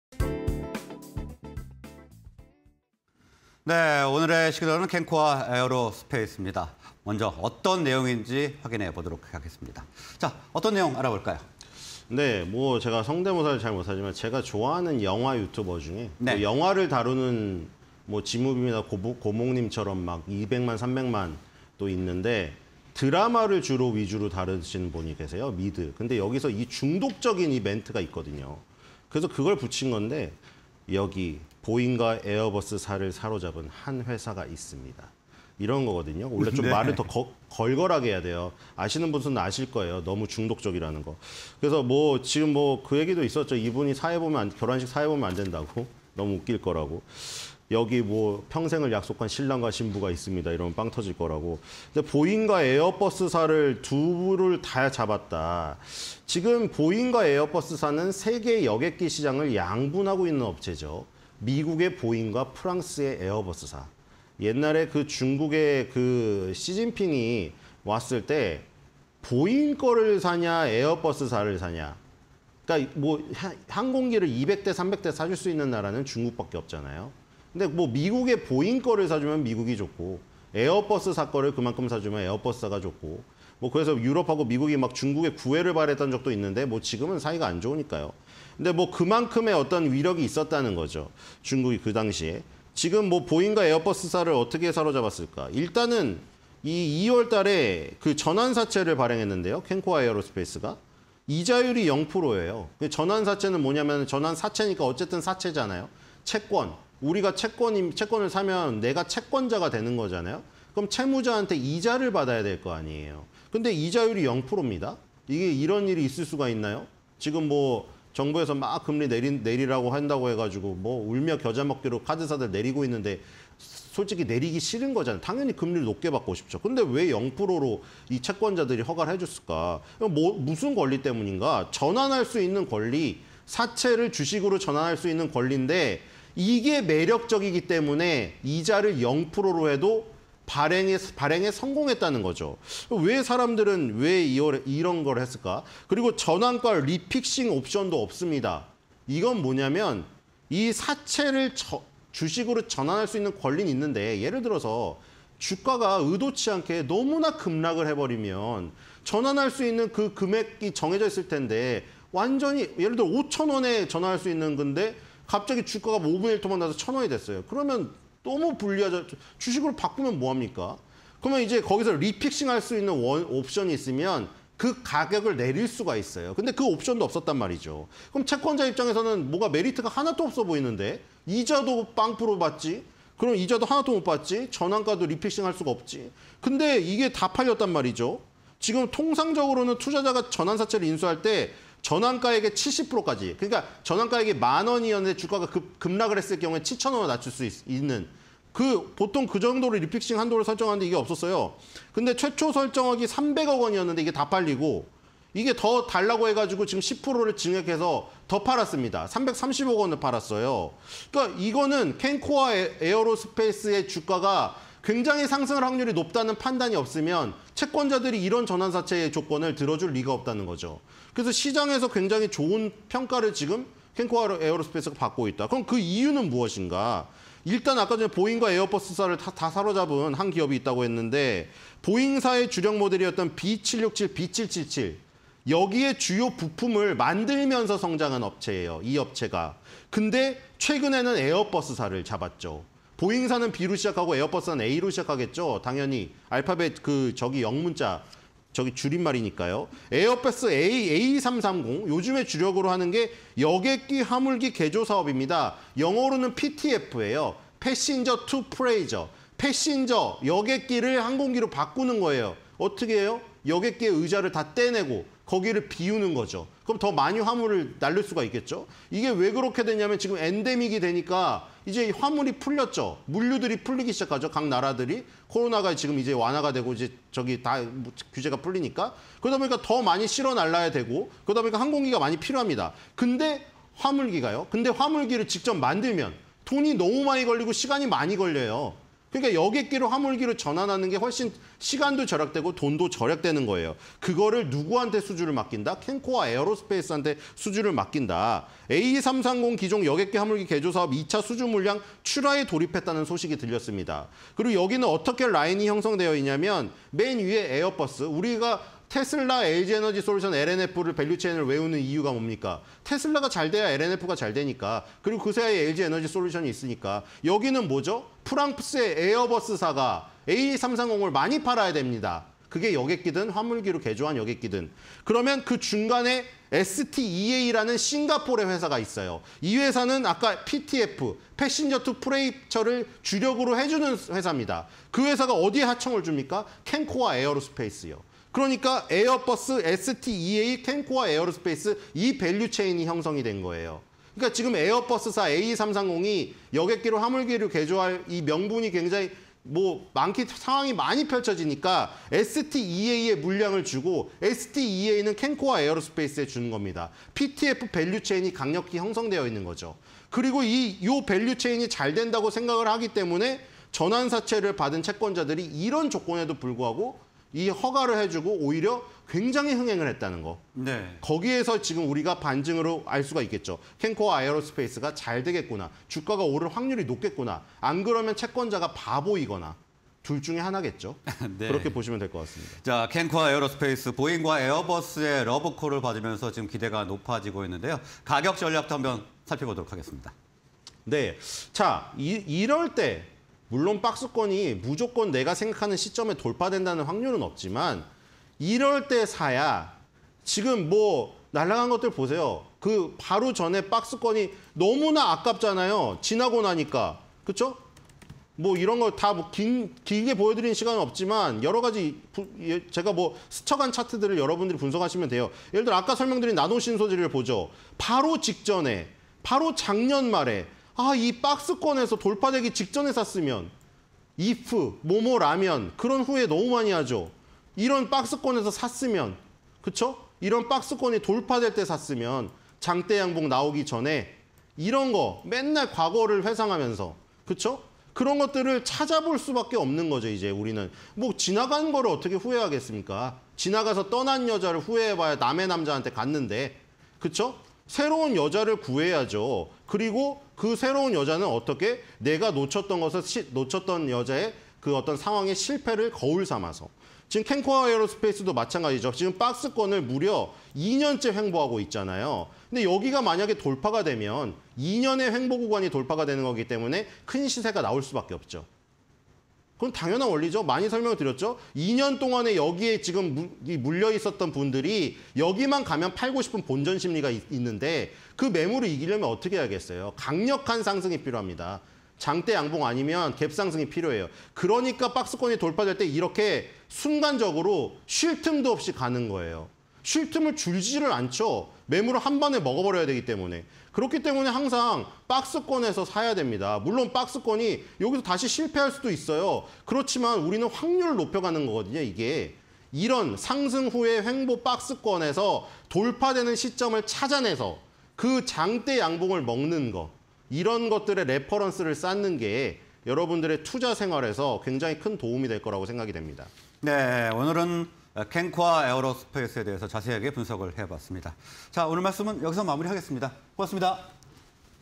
켄코아에어로스페이스(274090), 고성장 전망... "보잉과 에어버스를 사로잡았다"_신소장의 시그널 (20230308)
켄코아에어로스페이스(274090) "보잉·에어버스의 러브콜"
- 330억원 규모 CB 발행 성공… 전환가 리픽싱 옵션 無
- 美 보잉과 에어버스 등에 납품
- 에어버스-STEA-켄코아 등 PTF 밸류체인